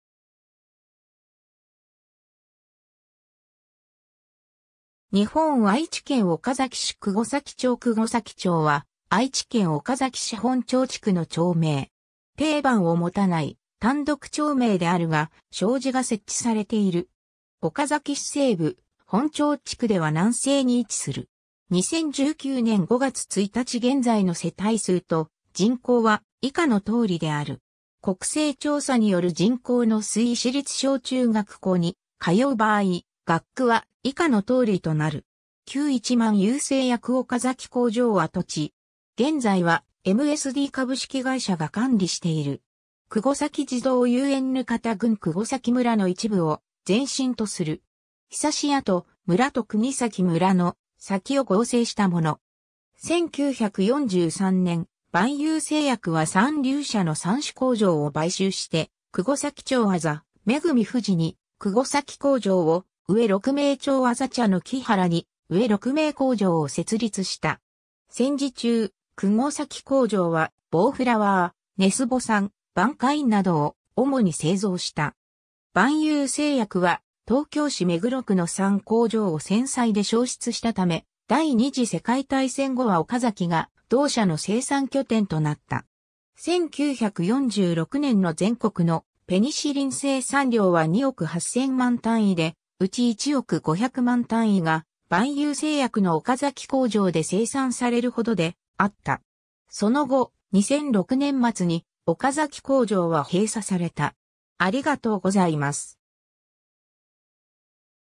久後崎町
岡崎市の町・字
日本 」 愛知県 」 岡崎市 」 久後崎町 久後崎町（くごさきちょう）は、愛知県岡崎市本庁地区の町名。
丁番を持たない単独町名であるが、小字が設置されている。
 岡崎市西部、本庁地区では南西に位置する。
 2019年（令和元年）5月1日現在の世帯数と人口は以下の通りである。
 国勢調査による人口の推移 市立小・中学校に通う場合、学区は以下の通りとなる。
 旧萬有製薬岡崎工場跡地。
現在はMSD株式会社が管理している。
 久後崎児童遊園 額田郡久後崎村の一部を前身とする。
 久後村と国崎村の「崎」を合成したもの。
 1943年（昭和18年）、万有製薬は三龍社の蚕種工場を買収して、久後崎町字恵藤に久後崎工場を、上六名町字茶の木原（現・上六名3丁目）に上六名工場を設立した。
戦時中、久後崎工場はボーフラワー、ネスボサン、バンカインなどを主に製造した。
万有製薬は東京市目黒区の3工場を戦災で焼失したため、第2次世界大戦後は岡崎が同社の生産拠点となった。
1946年（昭和21年）の全国のペニシリン生産量は2億8,000万単位で、うち1億500万単位が万有製薬の岡崎工場で生産されるほどであった。
その後、2006年（平成18年）末に岡崎工場は閉鎖された。